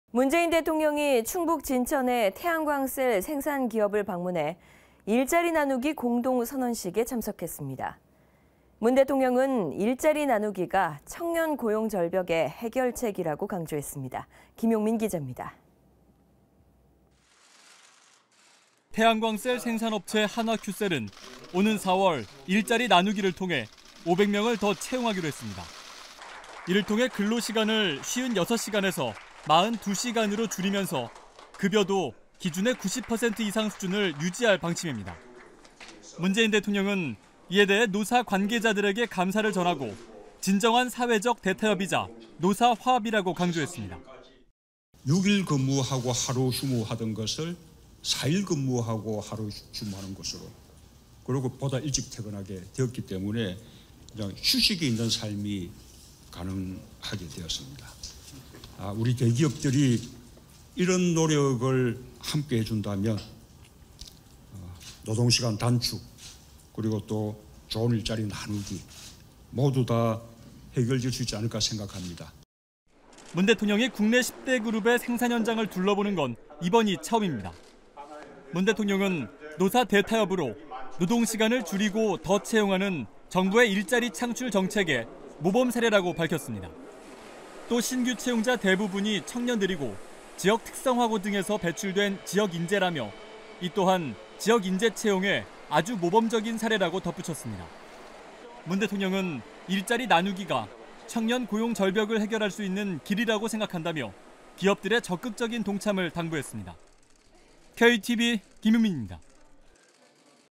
문재인 대통령"일자리 나누기, 청년 고용 절벽의 해결책"
문재인 대통령이 충북 진천의 태양광 셀 생산기업을 방문해 일자리 나누기 공동선언식에 참석했습니다.
문 대통령은 일자리 나누기가 청년 고용 절벽을 해결할 수 있는 길이라고 생각한다며 기업들의 적극적인 동참을 당부했습니다.